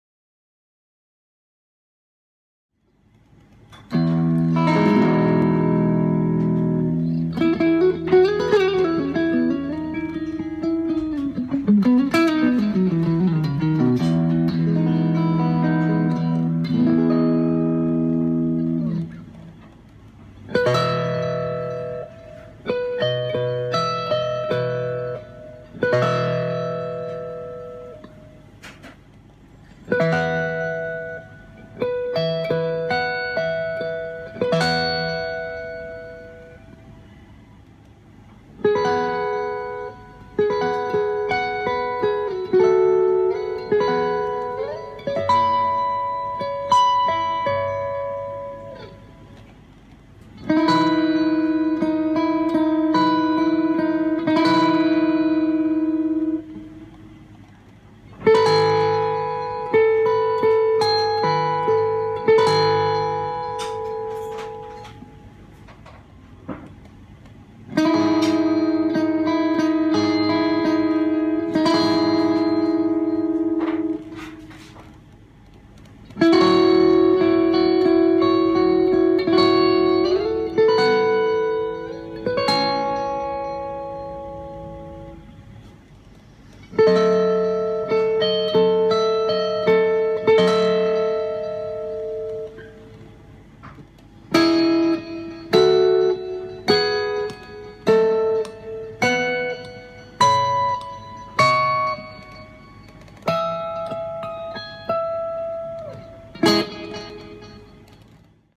【ギター】今日から使えるエモいボイシング
１弦２弦３弦を使用した個人的にかっこいいと思ったボイシングをまとめてみました。

過去にTwitterにアップした動画です。